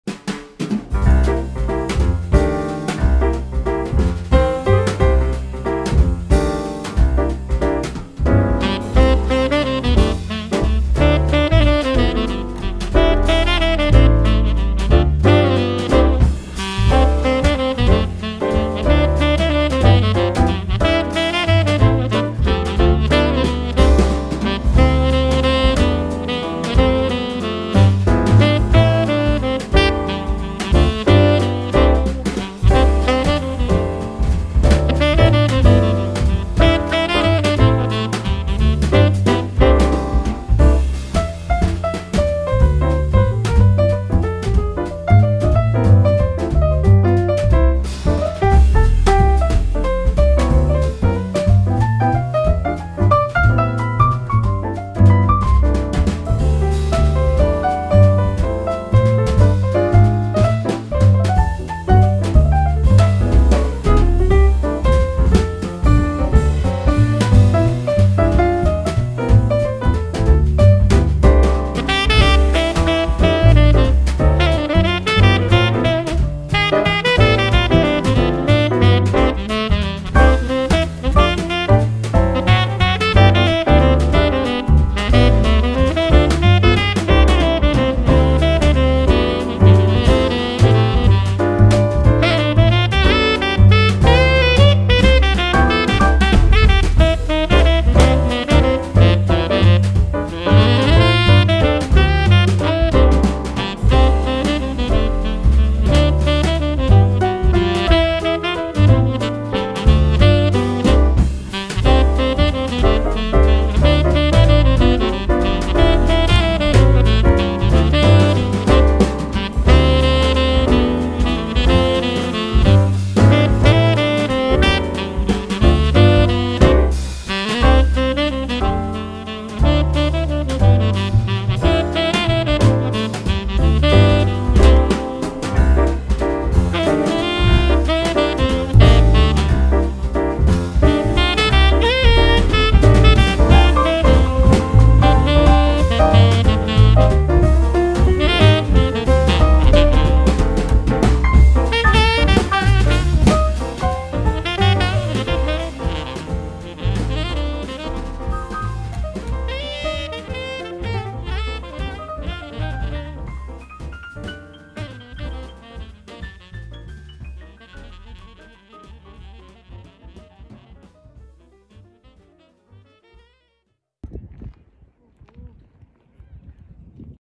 CHRISTMAS DAY BILTMORE ESTATE, Lots of snow! Asheville, NC | 4K Video
Asheville had a white Christmas this year! Short video showing Biltmore in the beautiful fluffy snow.